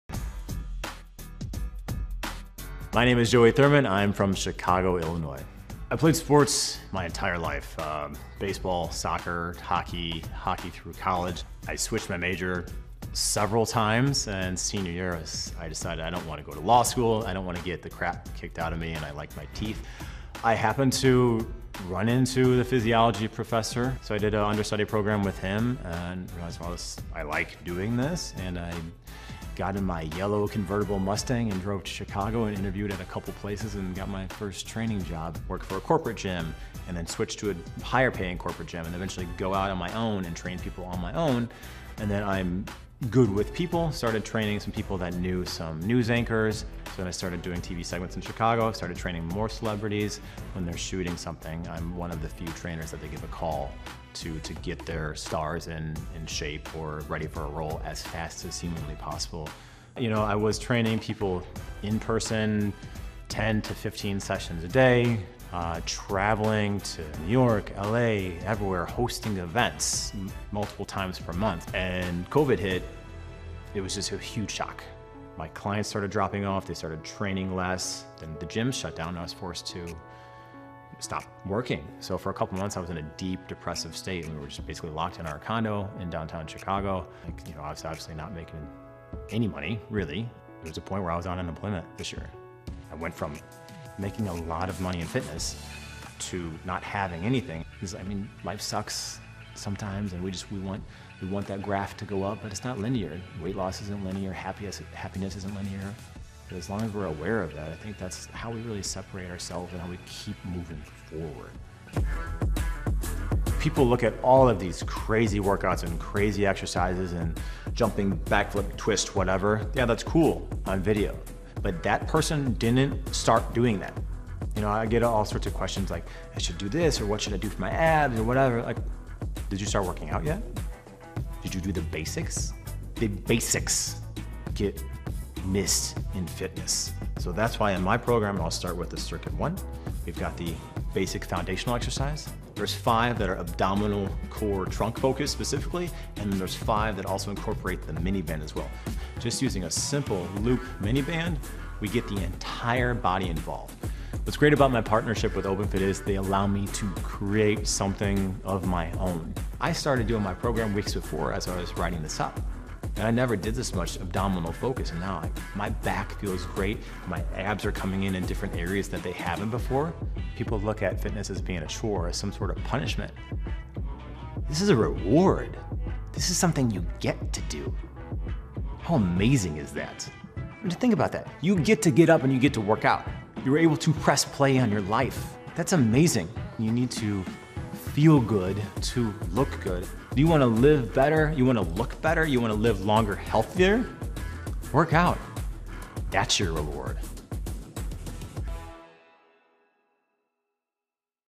"You Need to Feel Good to Look Good": Meet Openfit Trainer Joey Thurman
If you're looking for quick results, Joey Thurman is your go-to man. After all, it's his job to get TV and movie stars in-shape "as fast as humanly possible."

We caught up with Joey to learn more about what he thinks people get wrong about fitness and his basic, fundamental approach to training.

Follow Openfit for more fitness, nutrition, and wellness tips!